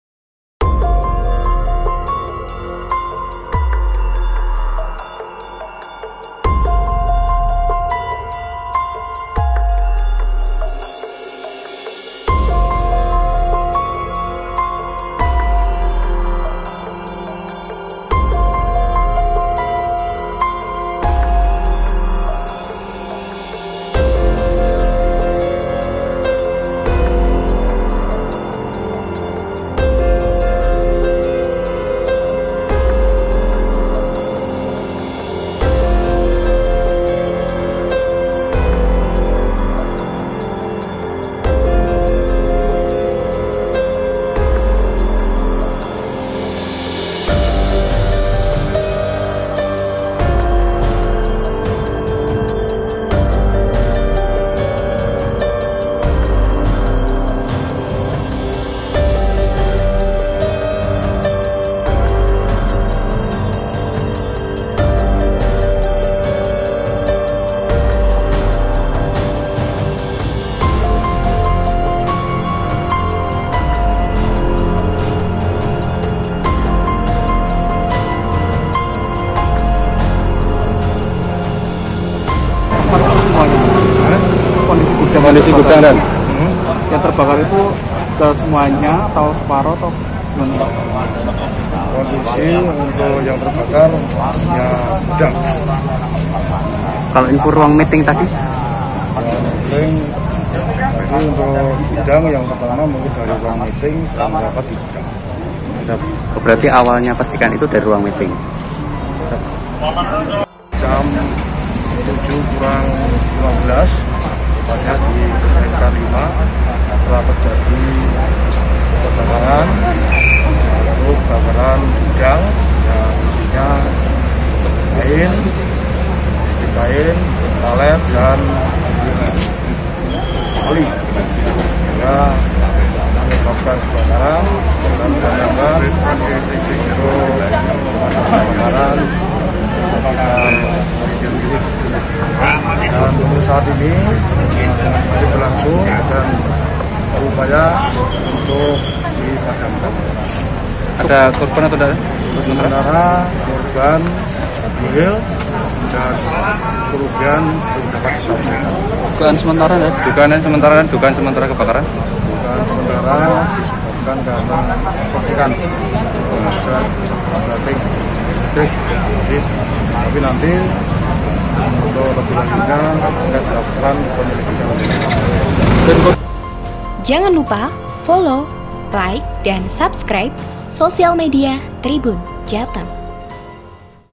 Kebakaran Di Pabrik Tekstil Sekar Lima Karanganyar
Kebakaran melanda pabrik tekstil Sekar Lima yang berada di Jalan Solo-Sragen tepatnya Desa Jetis Kecamatan Jaten Kab Karanganyar, sekira pukul 20.00.

Dari pantauan di lokasi, lima armada mobil pemadam kebakaran dari Damkar Satpol PP Karanganyar, BPBD dan Damkar Kota Solo dikerahkan untuk memadamkan kobaran api yang membakar